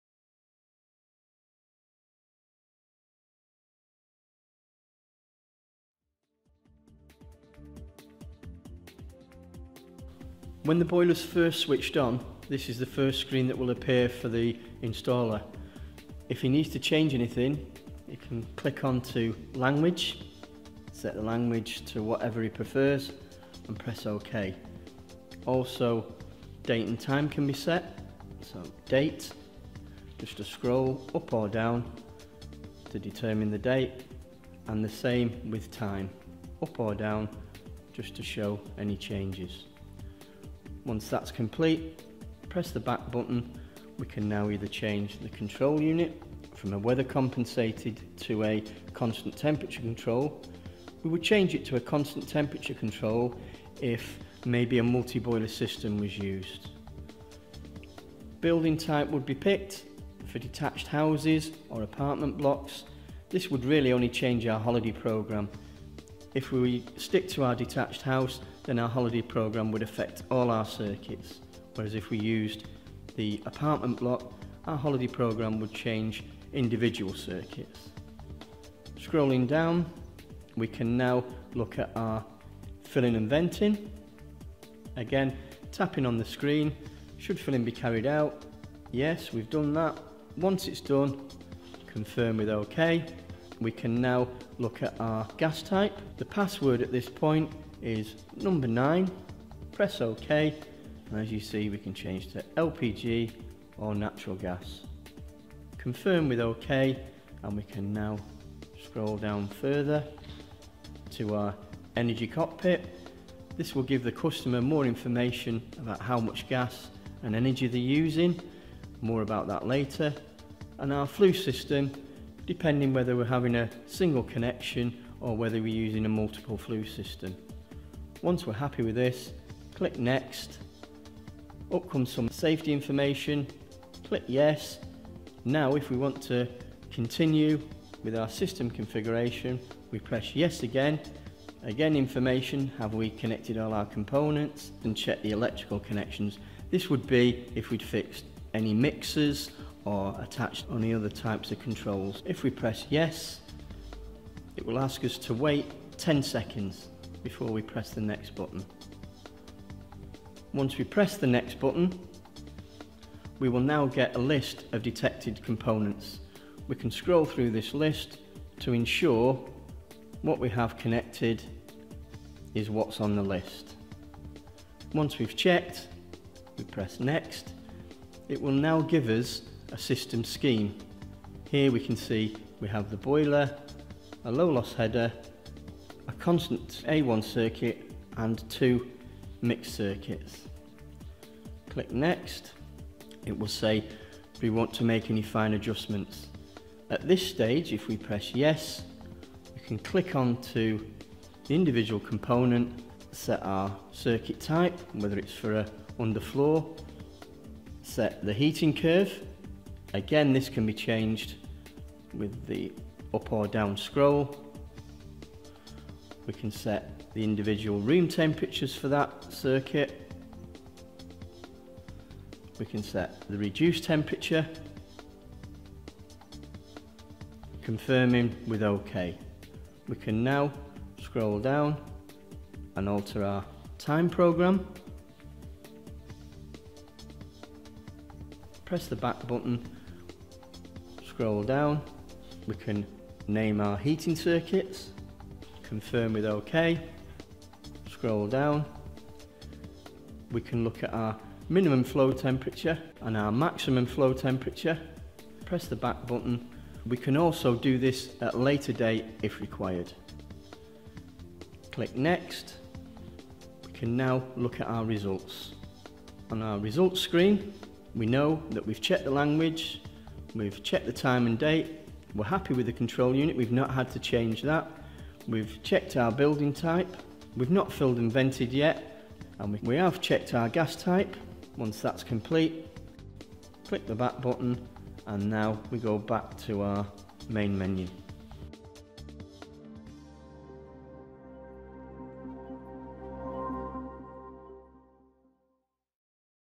Vitodens 200 W: How to set up Vitodens 200 W boiler commissioning menus
Viessmann explains how to set up Vitodens 200-W boiler commissioning menus in an easy, step-by-step guide.

The content in this video is intended only for registered gas installers.  Work on gas appliances should only be carried out by qualified operatives.